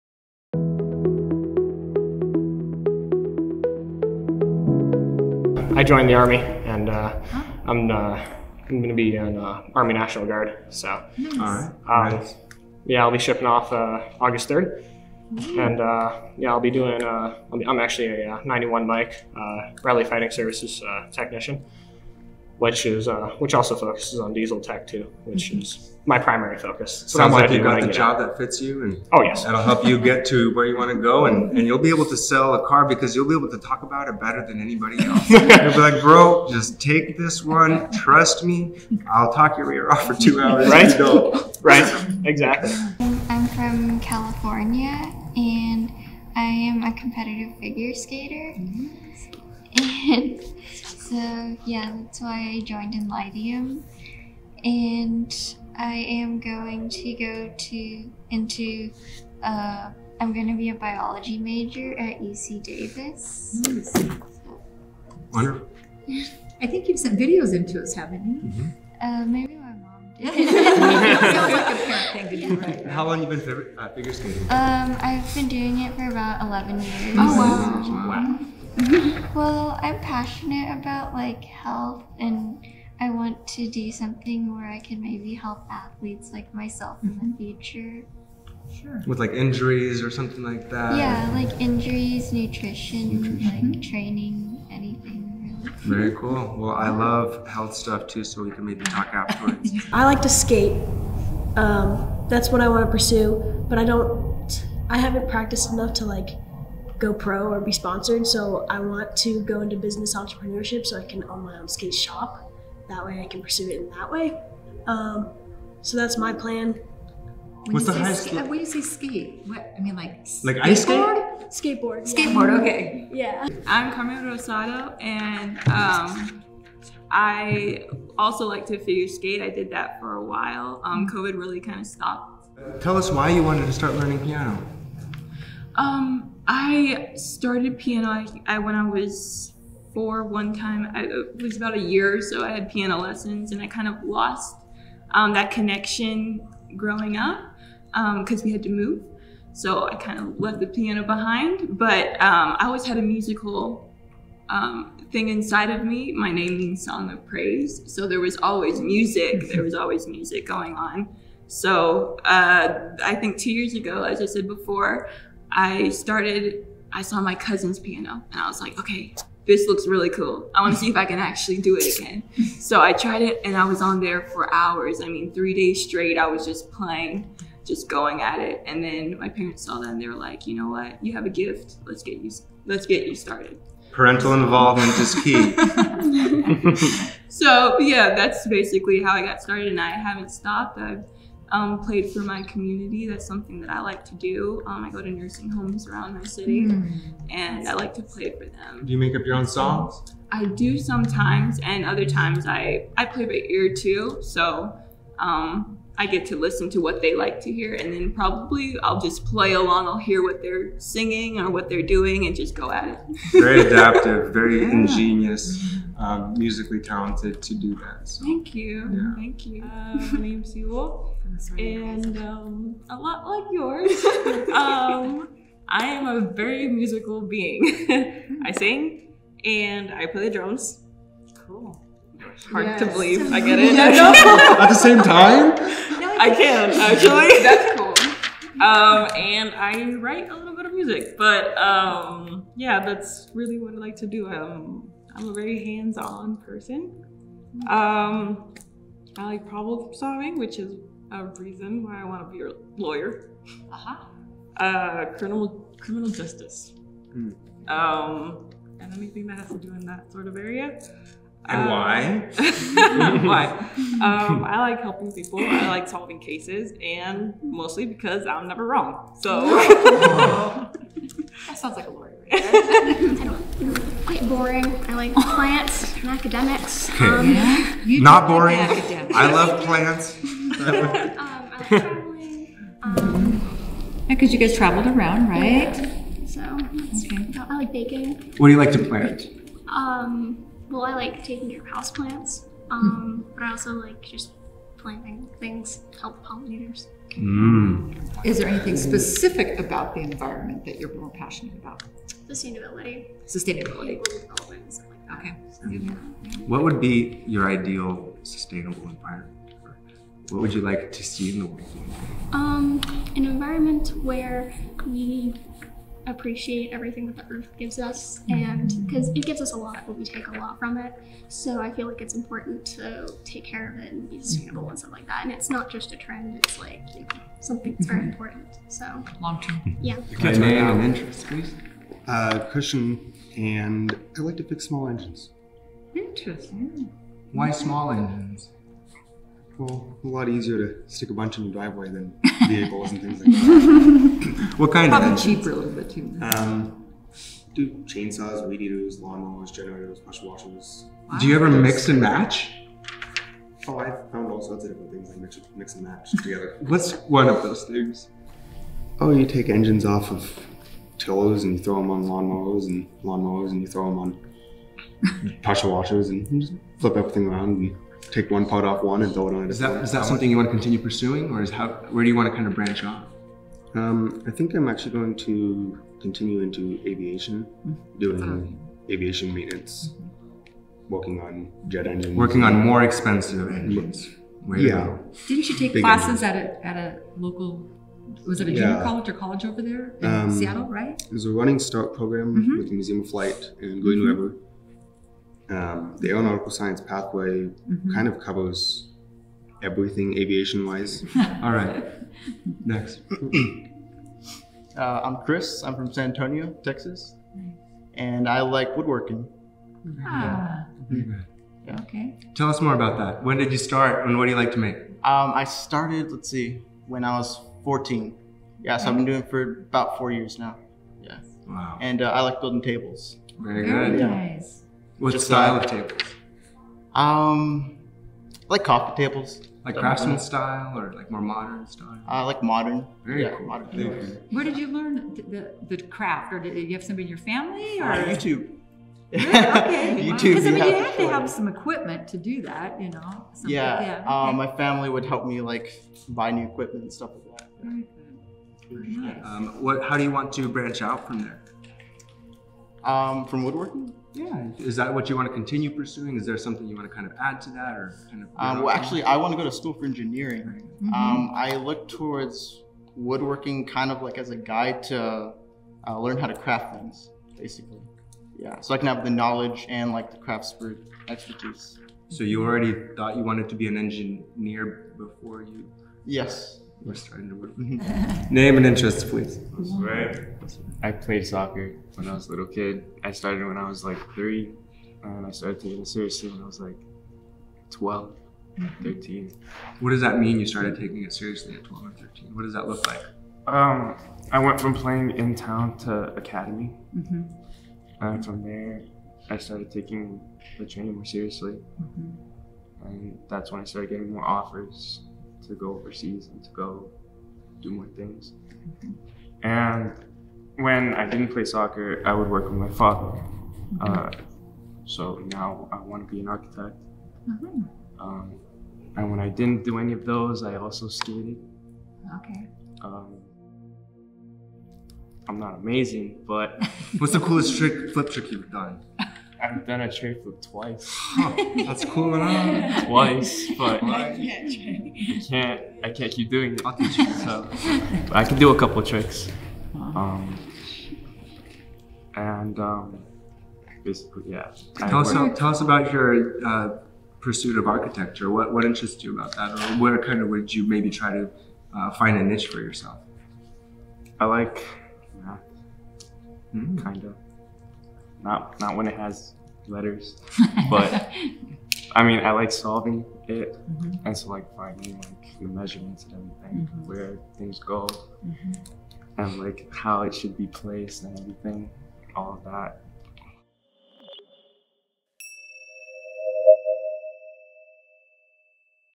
Episode 1 - Meet Enlightium Alumni - What are Your Interests
Listen to alumni graduates of Enlightium Academy share their interests, hobbies and goals in life together while meeting their classmates for the first time in person at their graduation event.  To learn more watch the other 4 episodes for more information on how to enroll your students and plan out their academic future for success.